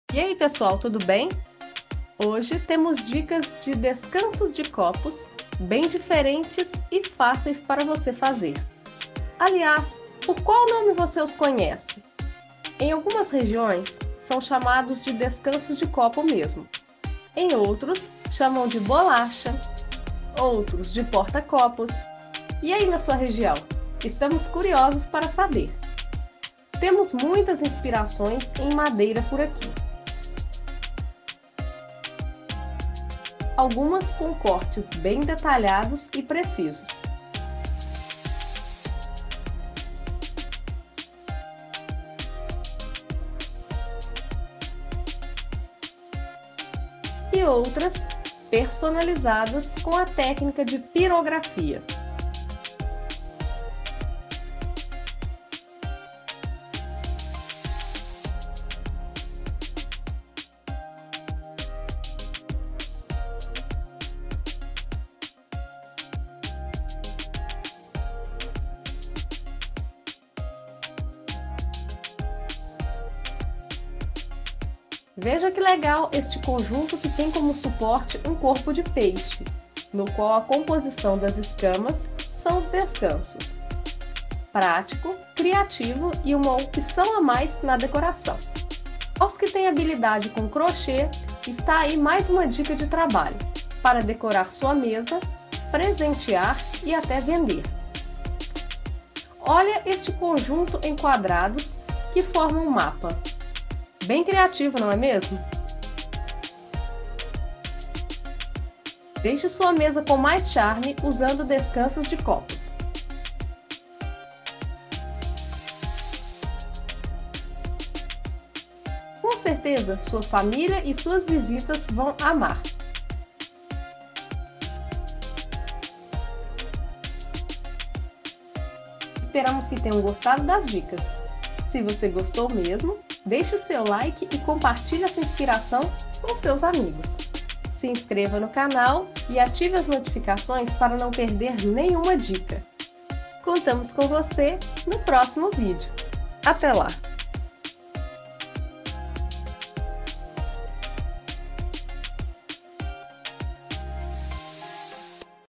12 Ideias de Porta de Copos lindos para você fazer e decorar cozinha
Assista ideias de porta copos. A verdade é que além de útil eles podem dar um charme na decoração. Então trouxemos ideias legais e diferentes e o que é melhor: fáceis de fazer. Um DIY bonito e barato para você se divertir fazendo.

Descansos de copos, bolacha, porta-copos, coasters, porta copos de madeira, DIY para decorar,